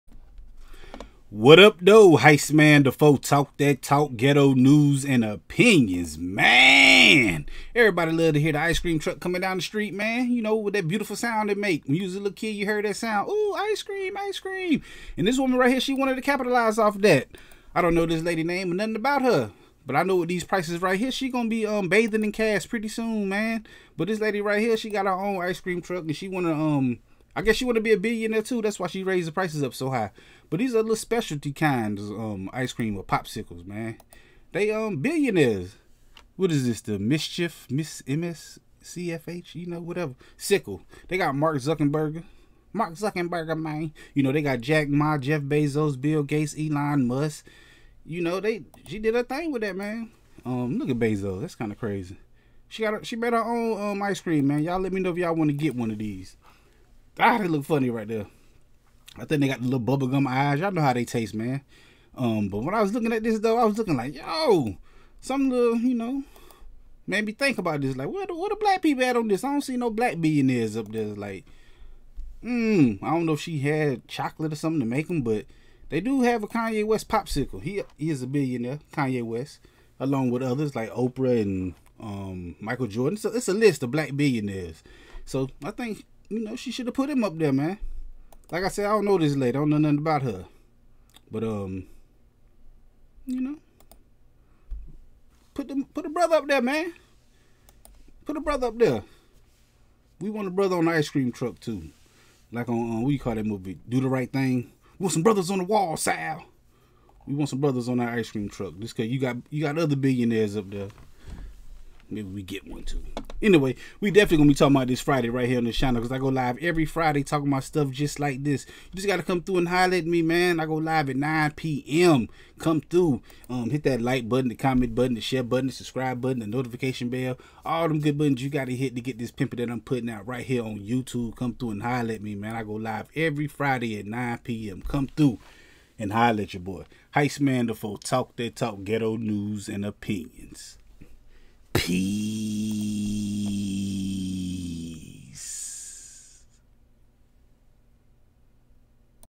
EAT THE RICH Ice Cream Truck Selling BILLIONAIRE Popsicles!
Gabriel Whaley and MSCHF, a Brooklyn-based art collective, sells billionaire themed popsicles from ice cream truck
Maaaaaaan!!!